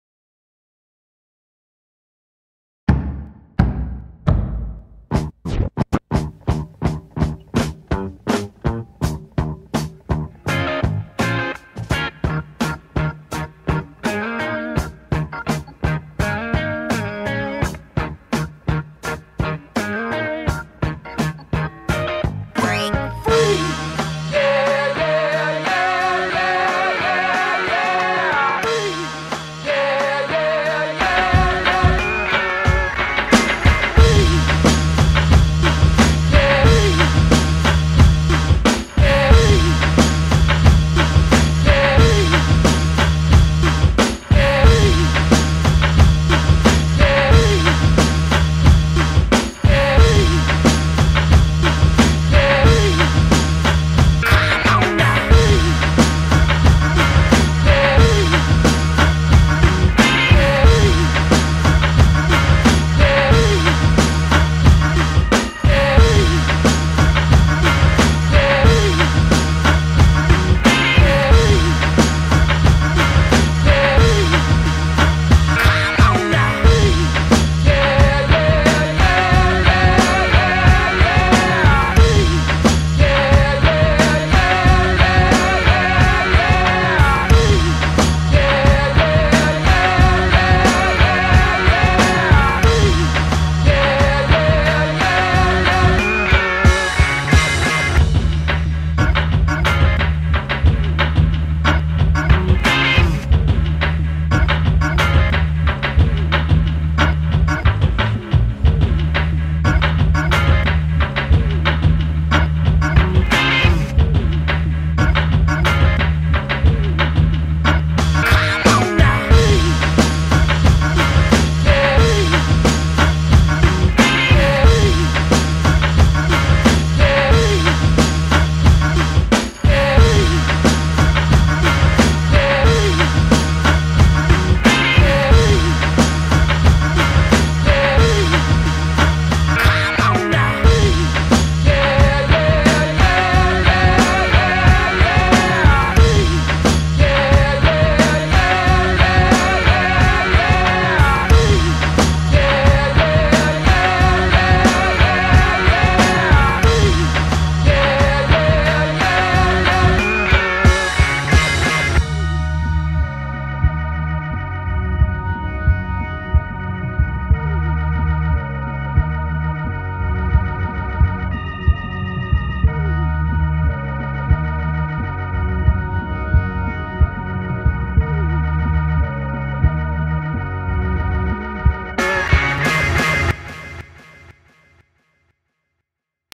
Jayteehazard - Behind The Decks
In 2010 I decided to starting taking photo's every time I got behind some turntables. These photo's are from roughly between January 2010 up until December 2012. Majority of shows featured here are with Last Kinection and Briggs.